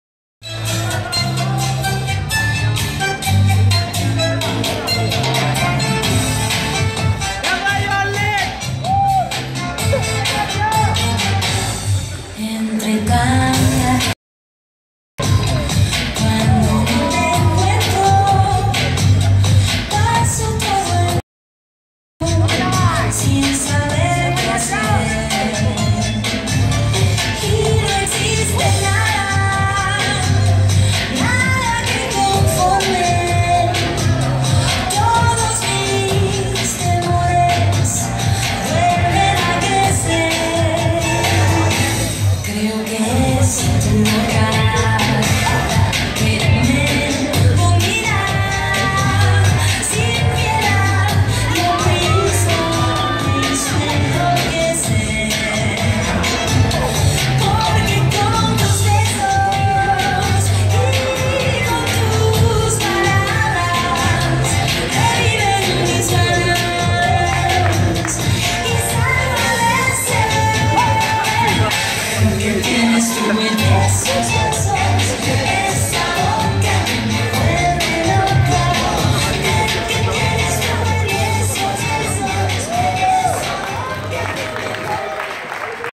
Finał GPP Dorośli Lider Lublin 2017 - Chacha / Magdalena Kogut & Aleksander Hołod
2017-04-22
Turniej tańca towarzyskiego. Grand prix Polski w kategorii Dorosłych. Organizowany przez szkołę tańca towarzyskiego "Lider" w Lublinie.